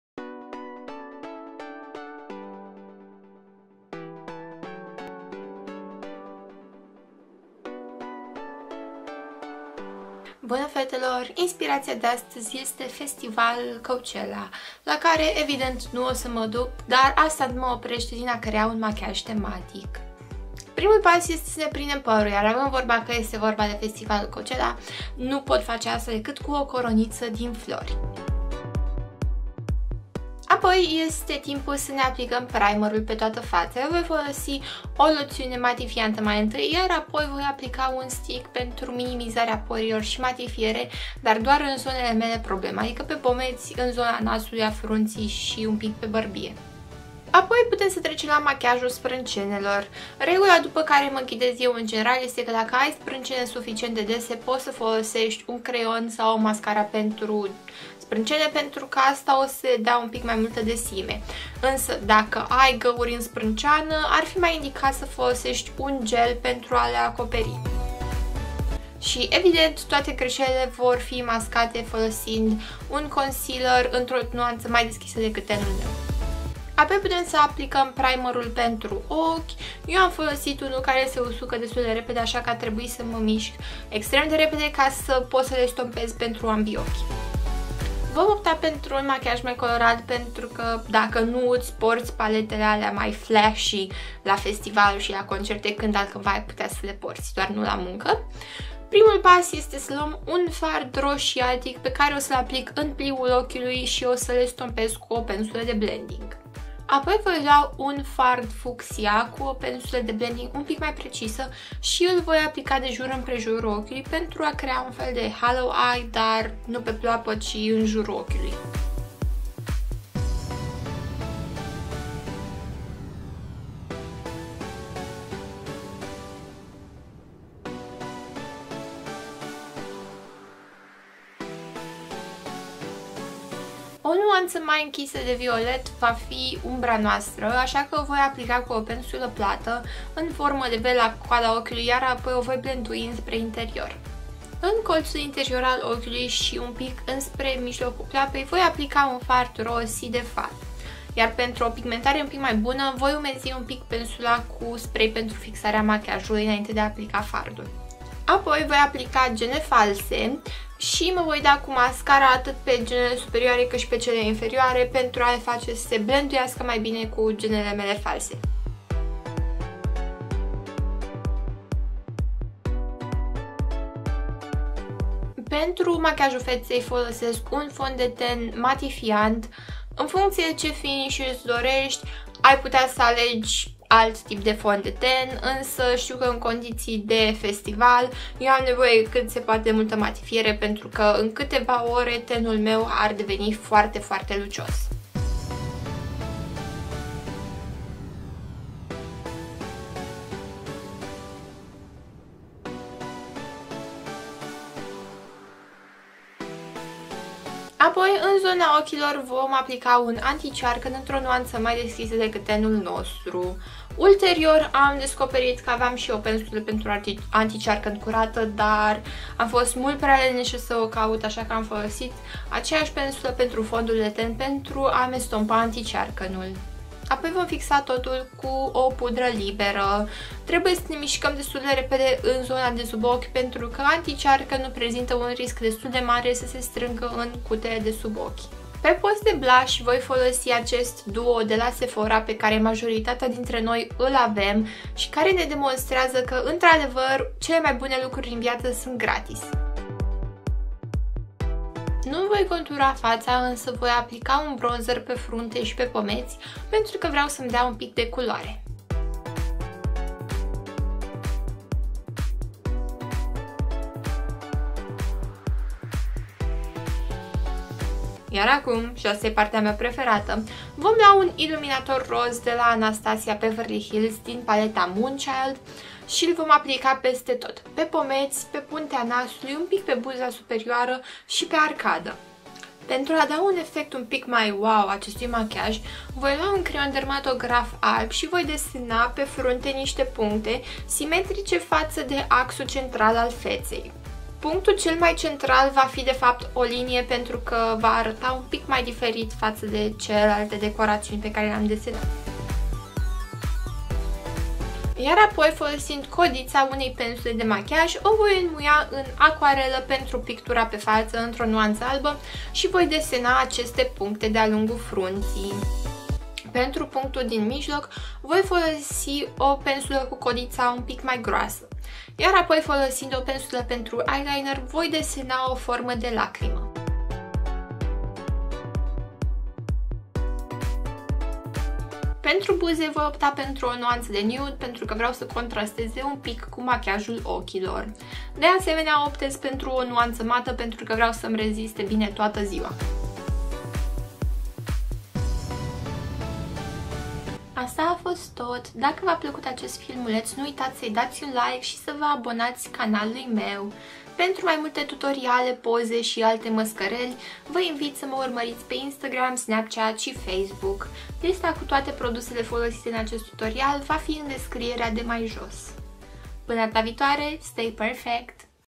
❤TUTORIAL❤ Festival Look// Coachella Inspired Makeup
Un machiaj nebun si colorat numai bun pentru festivaluri si concerte!

Stick matifiant Benefit the POREfessional license to blot
Creion sprancene bh Cosmetics Flawless brow
Primer pentru pleoape Benefit Air Patrol
Farduri de bleoape NYX Ultimate Brights & Cool Neutrals
Mascara Covergirl Plumpify Blastpro
Fond de ten Rimmel London Stay Matte
Anticearcan/ fond de ten mai deschis la culoare Clinique 2 in 1 Beyond Perfecting
Pudra Joko
Blush Sephora
Bronzer lÓreal Glam Bronze